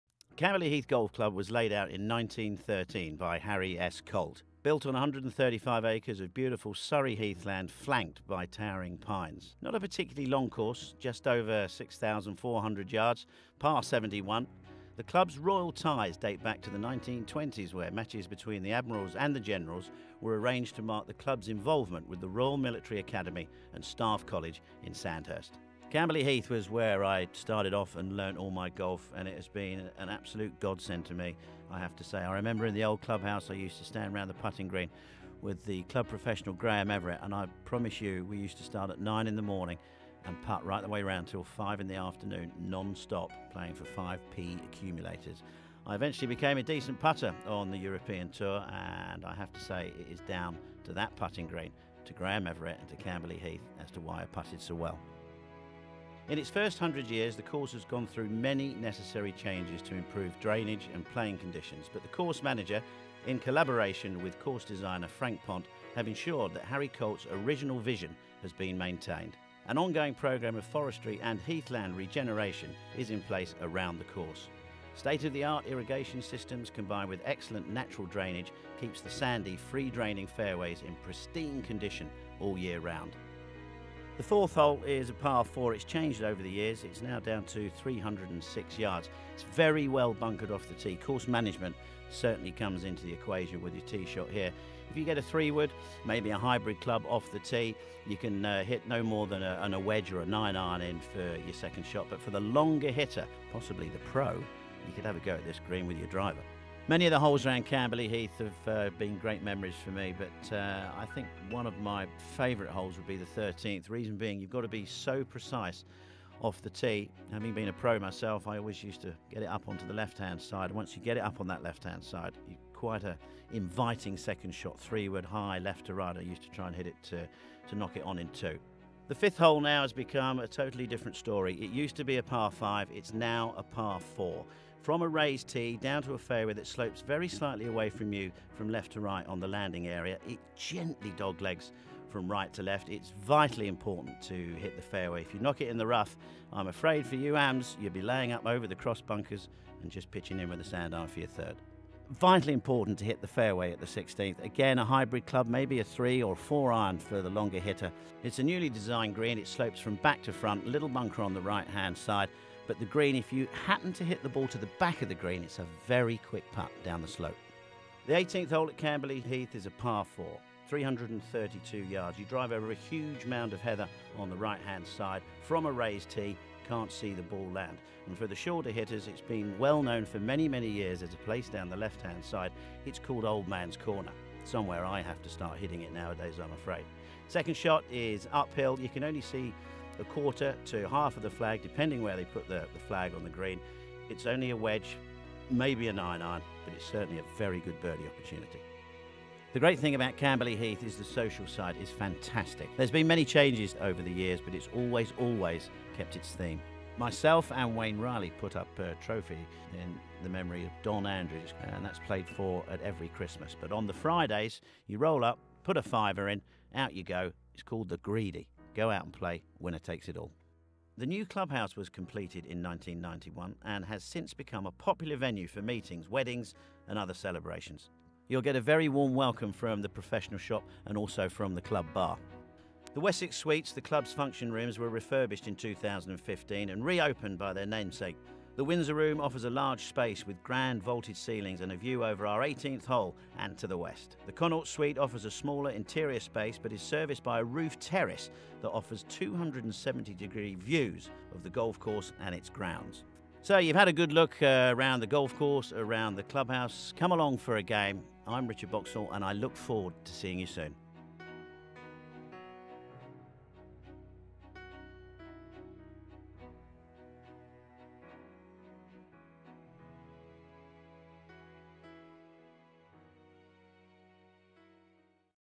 Camberley Heath Golf Club - An Introduction
Former European Tour Player and Voice of Sky Sports Golf; Richard Boxall gives us a taste of his home club, Camberley Heath.

Filming: Aerios Photography  /  Matt Link Photography